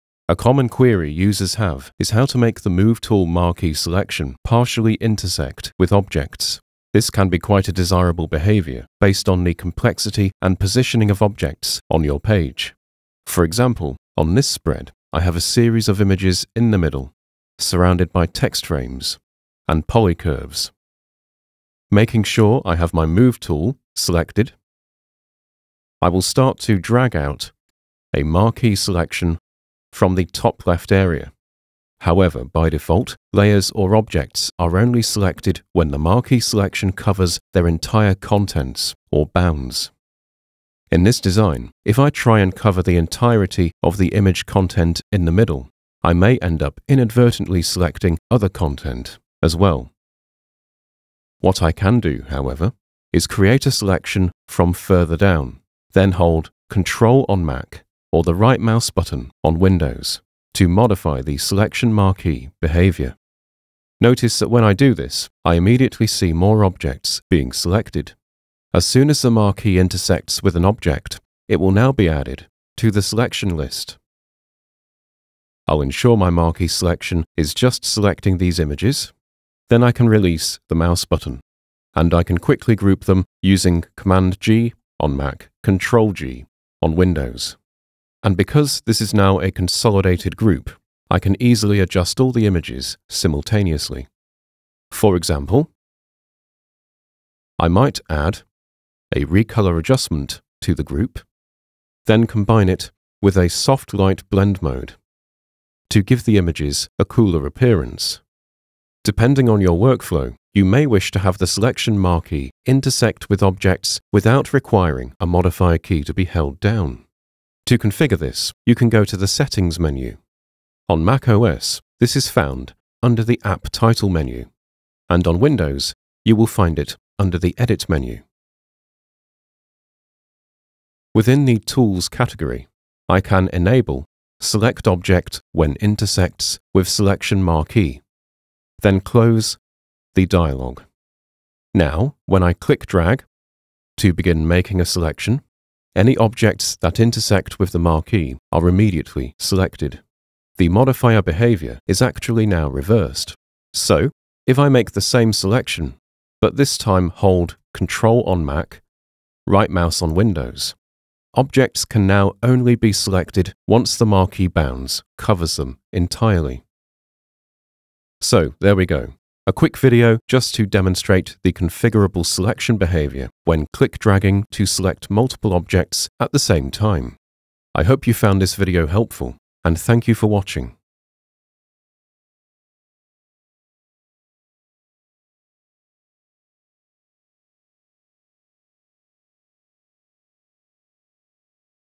Marquee Select Intersection (Affinity Publisher)
Discover an easy way to 'invert' the selection marquee behaviour so that it automatically selects objects as soon as the selection box reaches them. This video will be particularly beneficial to users coming from other software where the default selection behaviour is different.

High Notes design by Benjamin Viney.